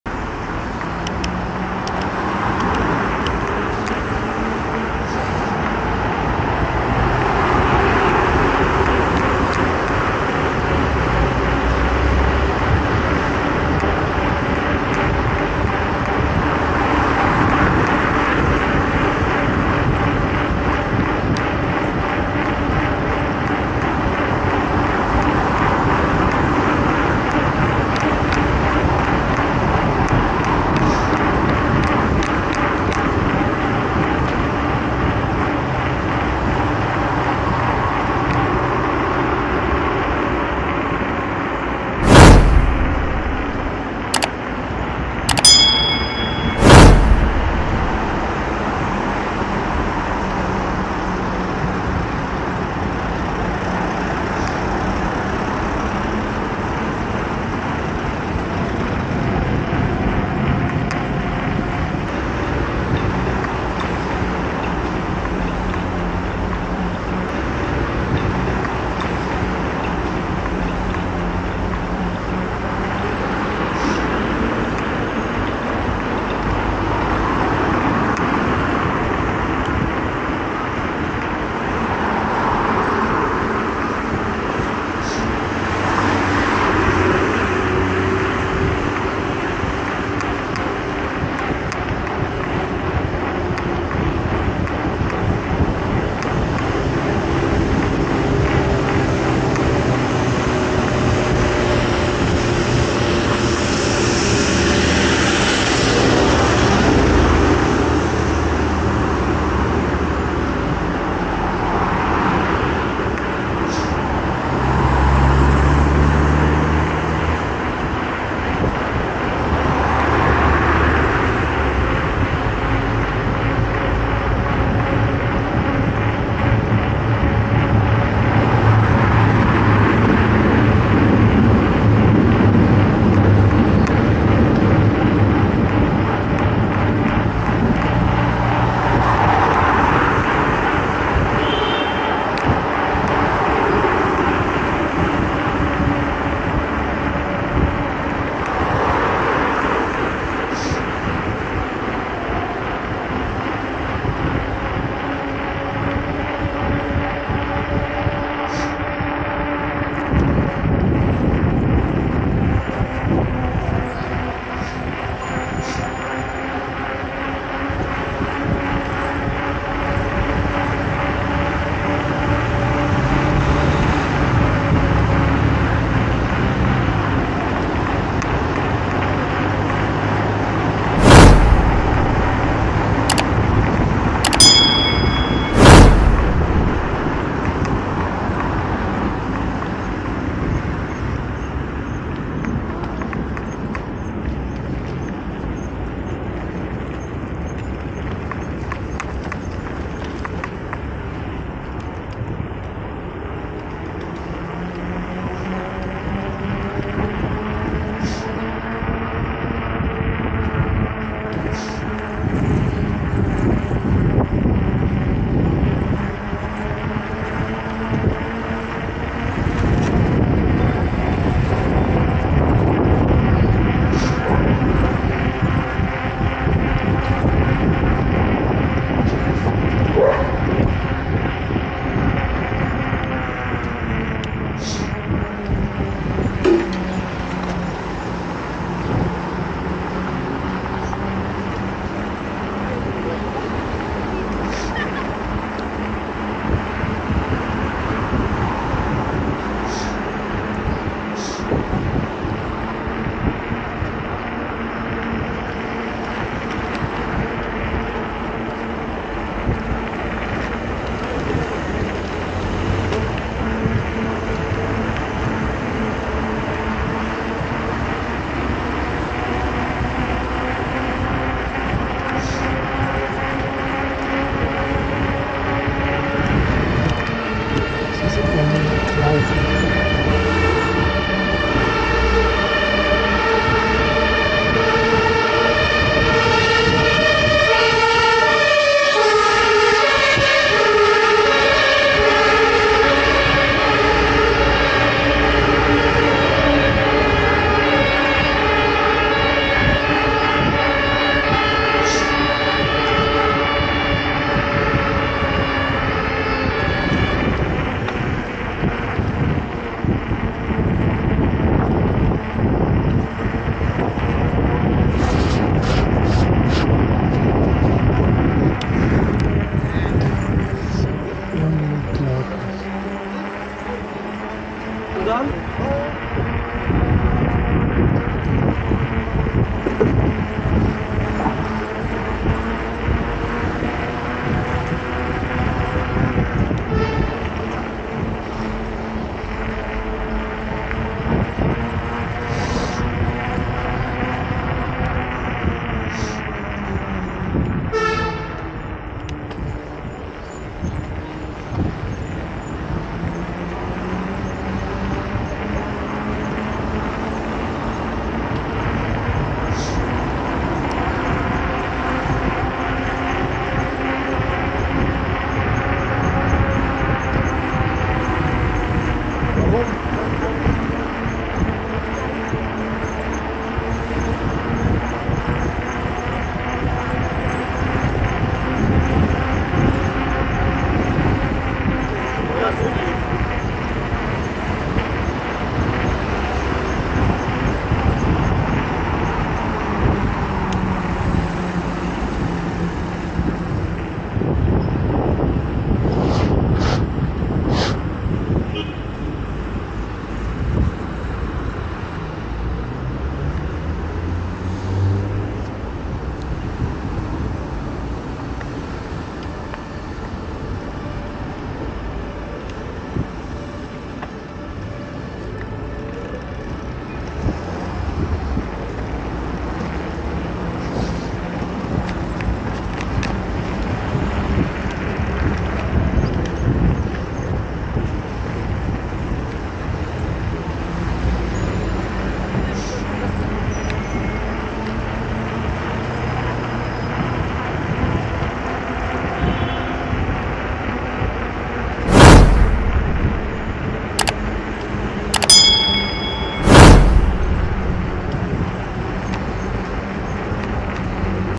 Berlin by bike & 4K -Vom European Patent Office Gitschiner Straße zum Hermannplatz
Berlin by bike & 4K -Vom European Patent Office Gitschiner Straße zum